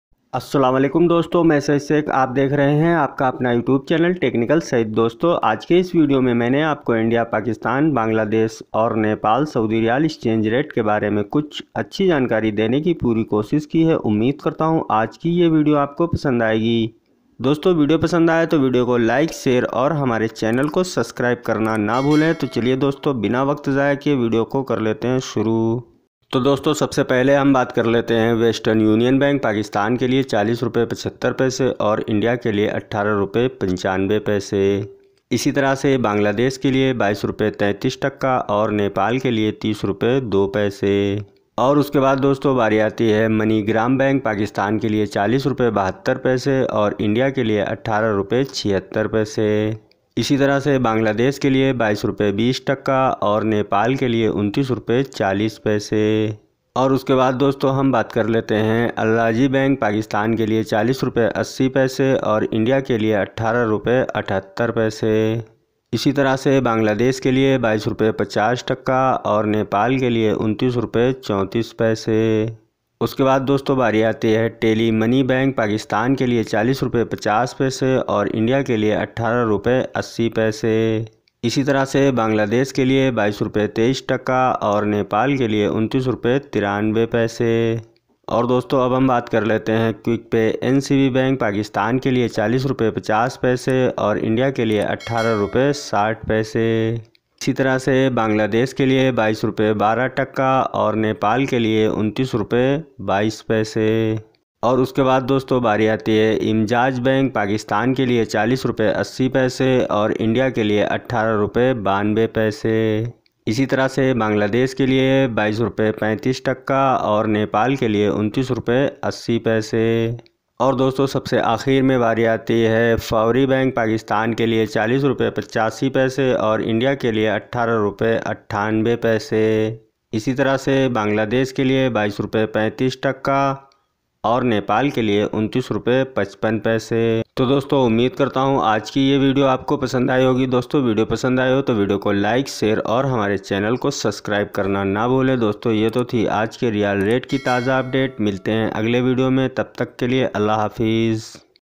AAJ KA REYAL RATE | India pakistan Bangladesh aur Nepal saudi riyal rate | today Saudi riyal
Saudi riyal exchange rate today.in India Bangladesh Pakistan Nepal aaj ka riyal rate YouTube videos | 2019

Hi

Doston is video mein aap logo ko Saudi riyal exchange rate. India Pakistan Bangladesh aur Nepal exchange rate ke bare mein kuch achi jankari Dene ki puri koshish ki hai.